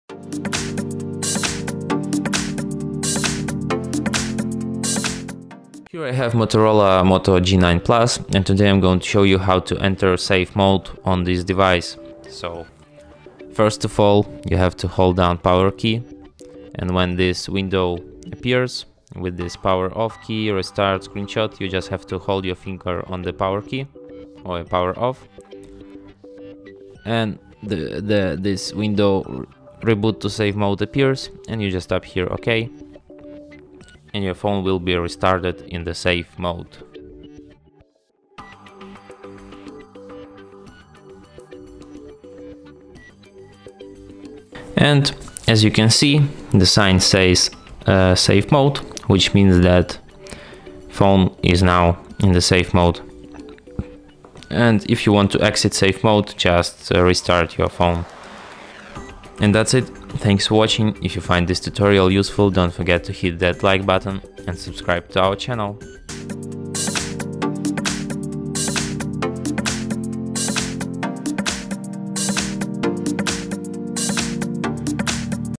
Safe Mode in MOTOROLA Moto G9 Plus – Diagnose Issues with Third-Party Apps
Learn more info about MOTOROLA Moto G9 Plus:
Find out the attached tutorial, where we teach you how to resolve the problem with third-party apps in MOTOROLA Moto G9 Plus. If you having some issues with installed apps on your Motorola device, stay with us to learn how to enable safe mode and diagnose the problem easily. Let’s follow the presented instructions and learn how to open, use, and exit diagnostic mode in MOTOROLA Moto G9 Plus. Visit our HardReset.info YT channel and discover many useful tutorials for MOTOROLA Moto G9 Plus.